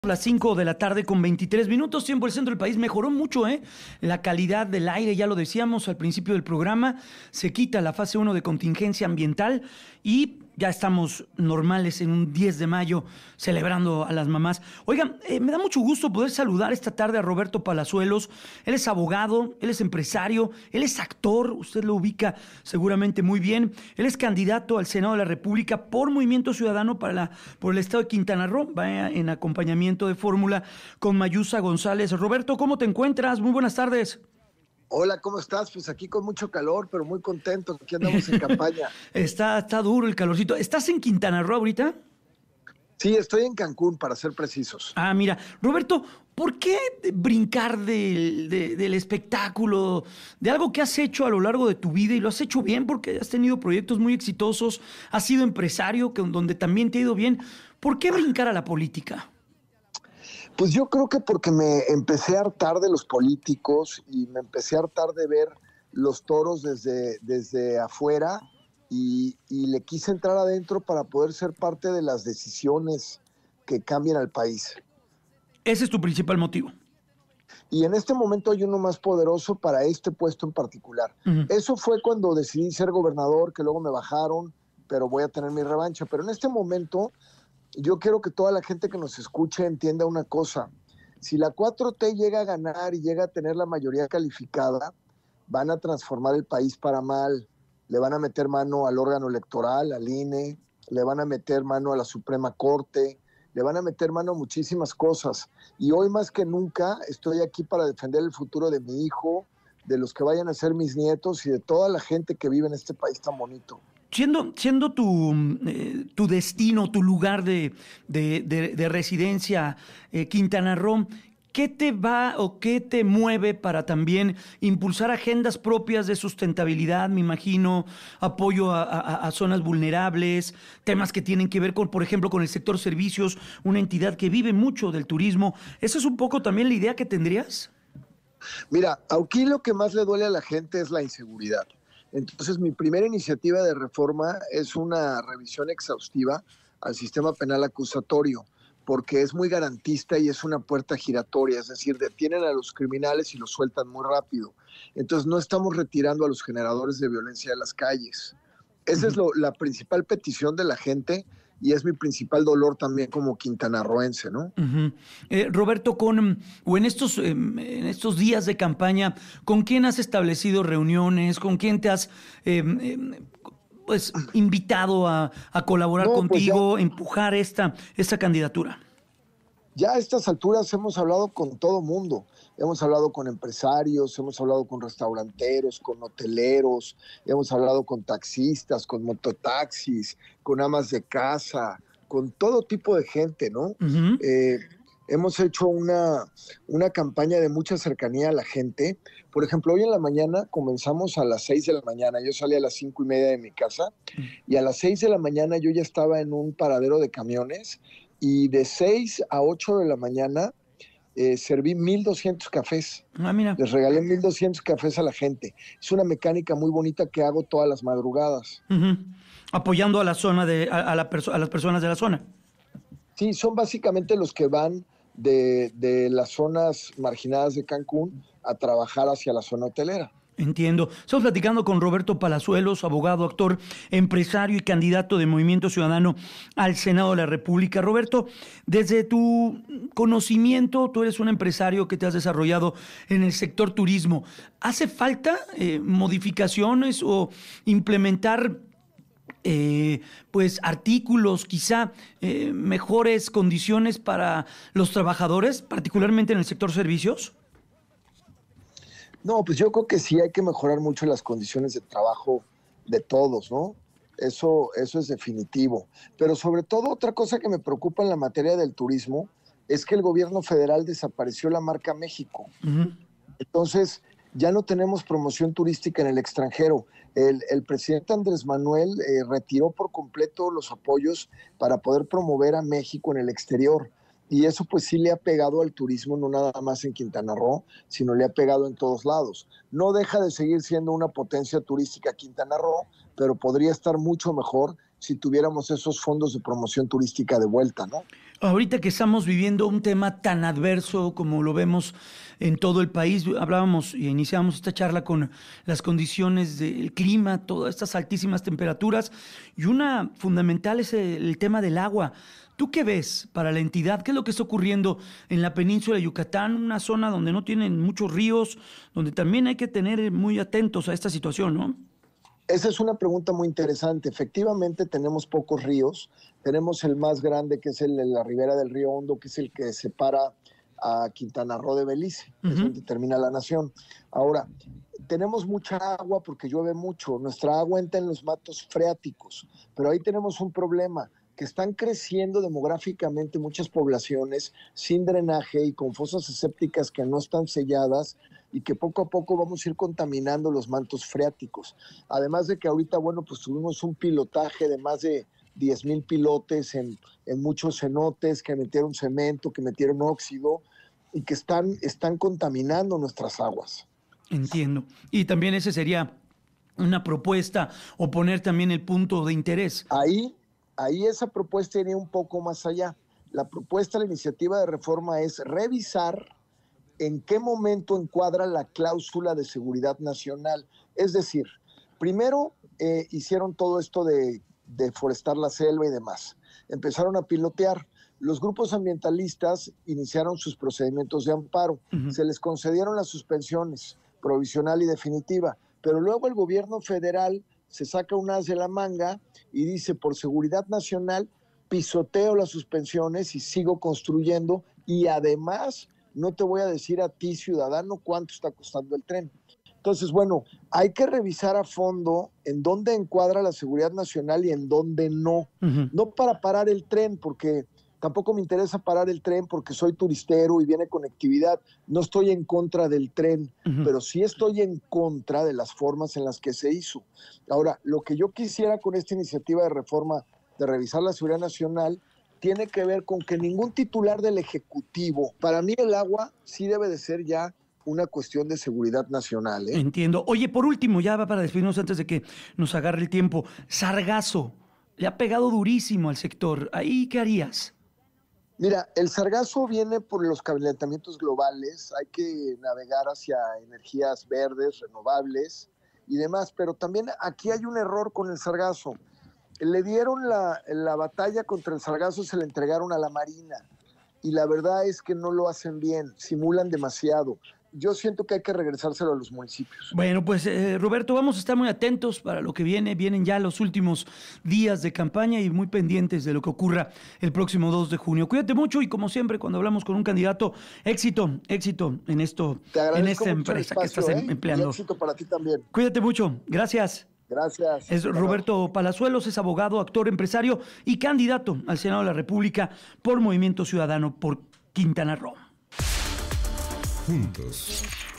Roberto Palazuelos asegura que no está en contra del Tren Maya
Roberto Palazuelos, actor y candidato al Senado en Quintana Roo por Movimiento Ciudadano, habló de las iniciativas que implementaría como senador. Busca fortalecer el turismo en toda la zona mejorando las condiciones de trabajadores. Enfatizó que no está en contra del Tren Maya ya que ha ayudado mucho a la conectividad, sin embargo, sí está en contra de la forma en la que se llevó a cabo.
Grupo Fórmula